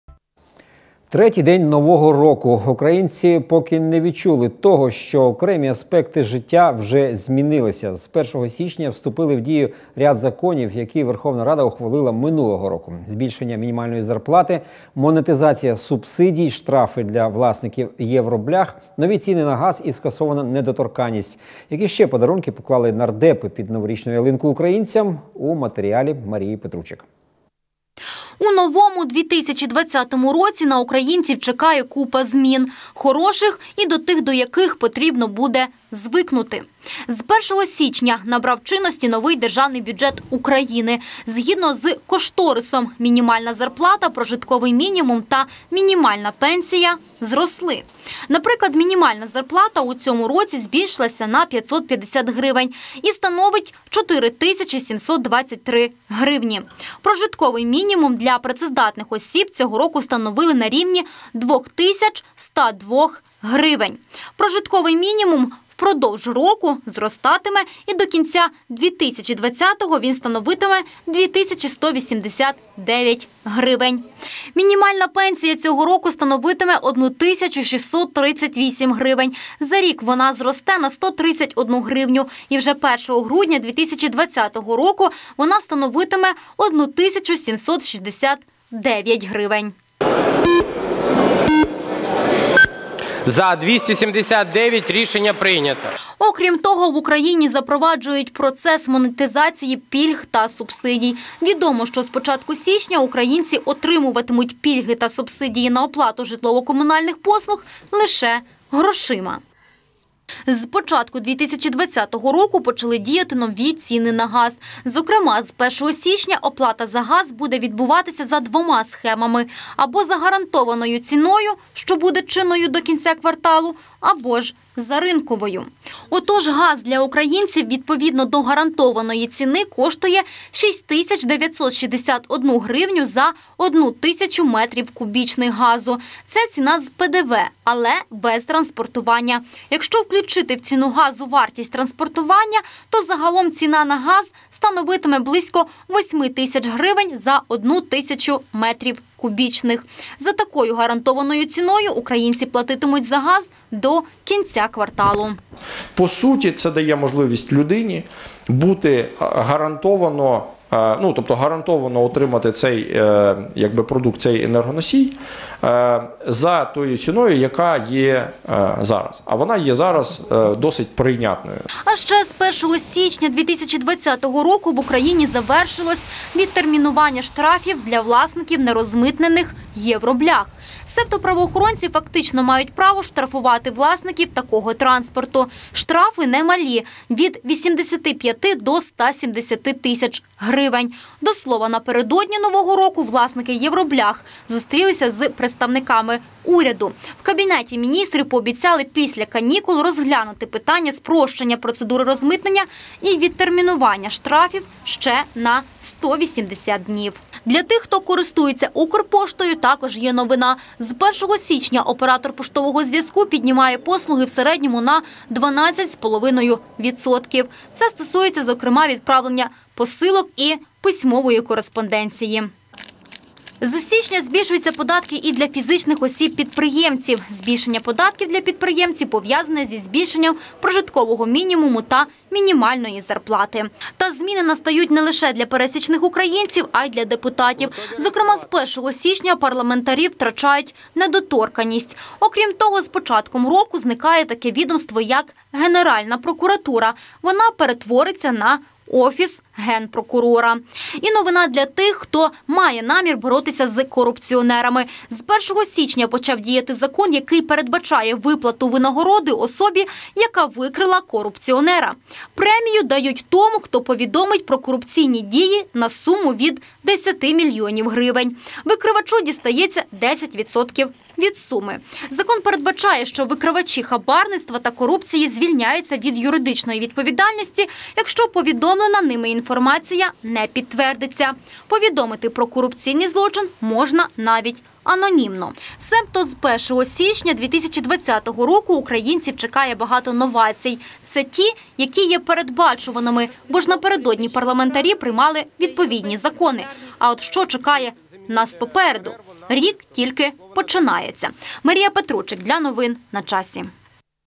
Які подарунки поклали нардепи під новорічну ялинку українцям
Третій день нового року. Українці поки цього не відчули, але їх життя вже змінилося. З першого січня вступили в дію ряд законів, які верховна рада прийняла минулого року. Збільшення мінімальної зарплати, монетизація субсидій, штрафи для власників «євроблях», нові ціни на газ, скасована недоторканість. Більше новин дивіться на нашому ю-туб каналі